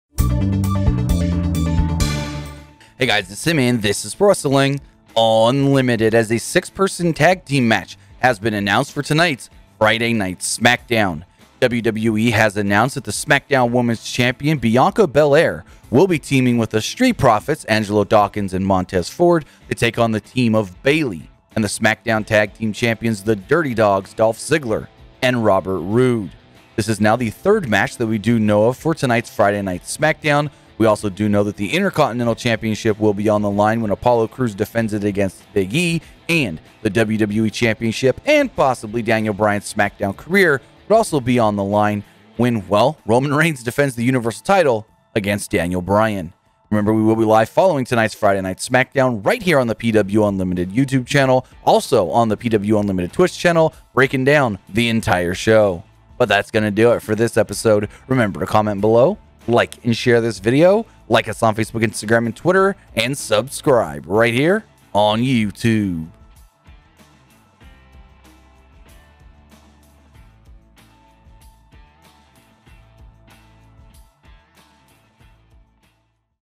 New Match Announced For Tonight's Friday Night Smackdown
With no long before tonight's Smackdown, WWE has announced the third match taking place on tonight's show.